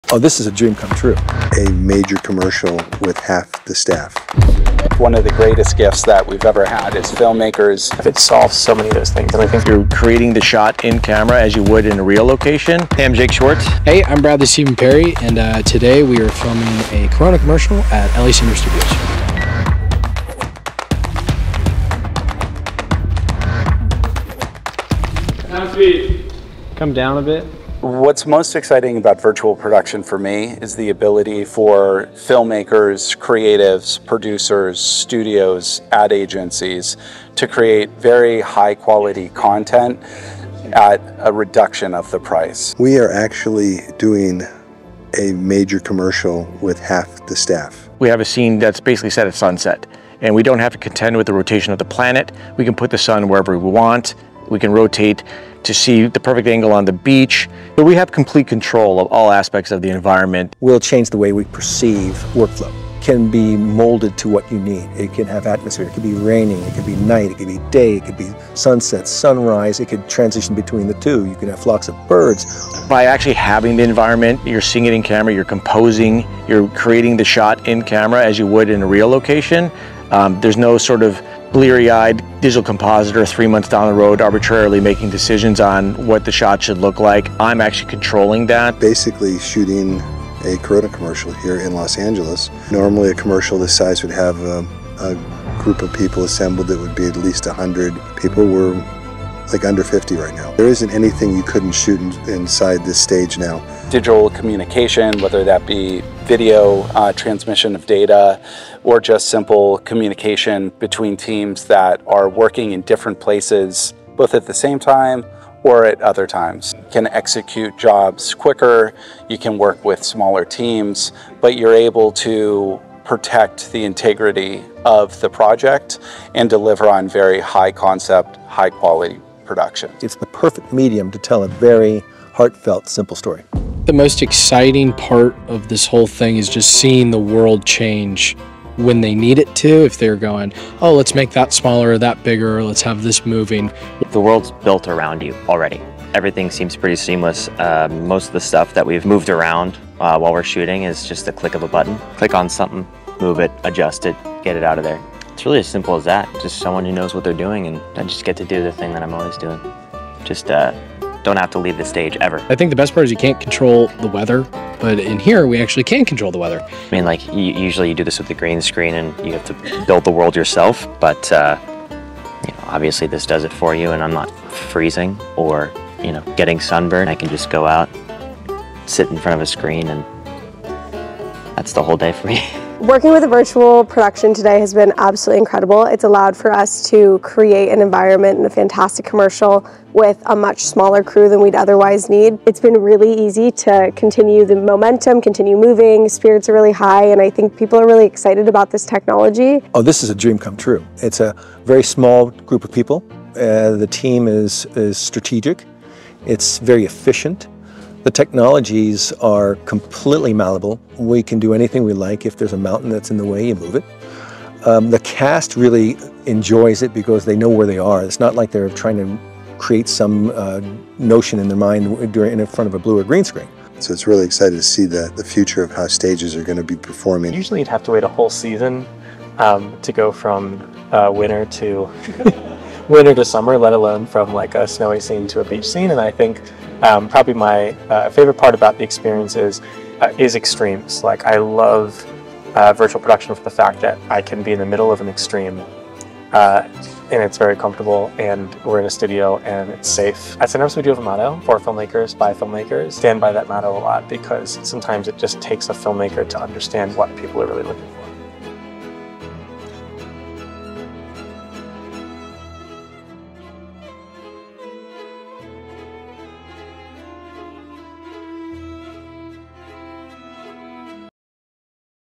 Virtual Technology Will Change Filmmaking Forever (Here's How...)
This behind the scenes tape was created to show the powerful virtual technology that is coming to take over the future of movie production. This Corona commercial was a spec advertisement.